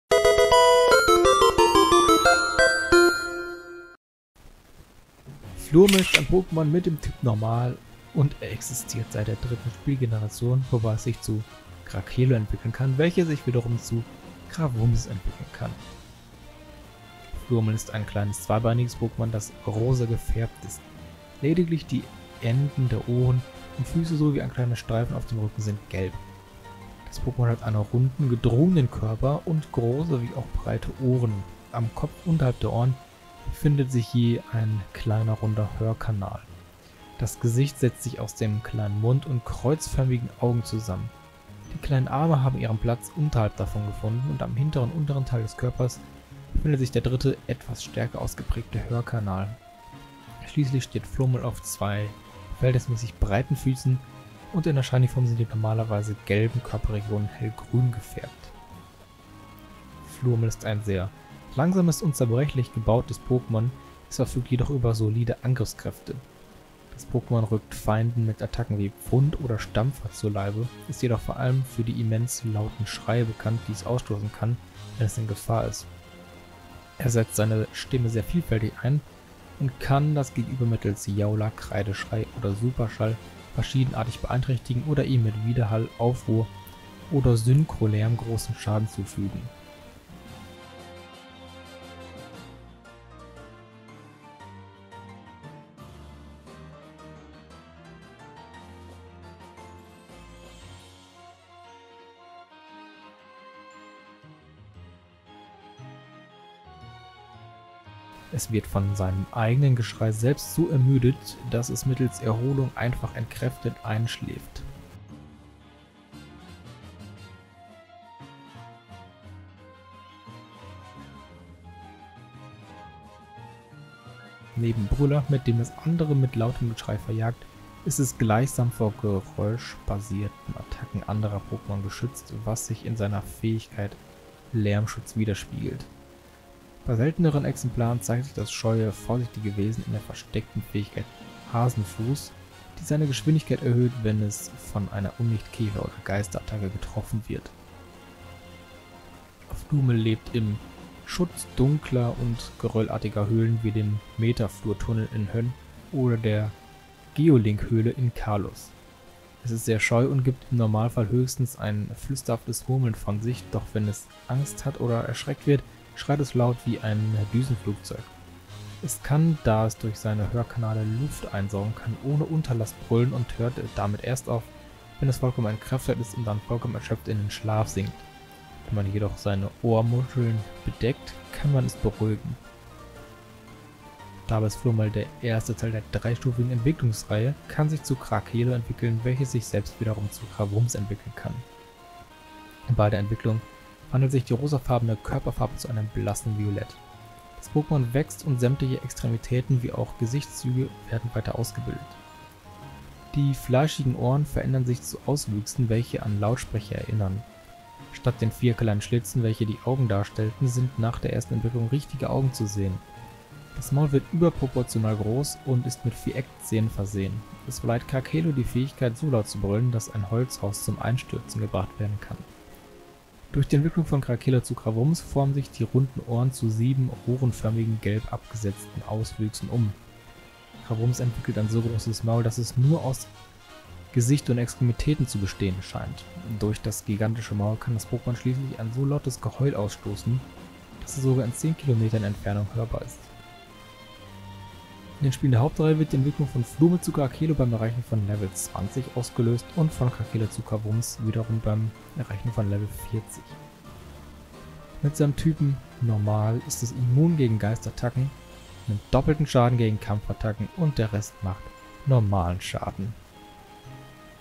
Das Pokémon #293 | Flurmel
▶️Worum gehts in diesem Video:

Ich stelle euch das Pokémon Flurmel vor. Dabei schauen wir auf das Aussehen, Attacken, Verhalten, Lebensraum, Entwicklung und Typ Schwächen. Viel Spaß :D

0:00 Intro
0:17 Aussehen
1:00 Attacken & Fähigkeiten
2:29 Verhalten & Lebensraum
3:03 Entwicklung
4:14 Typ Schwächen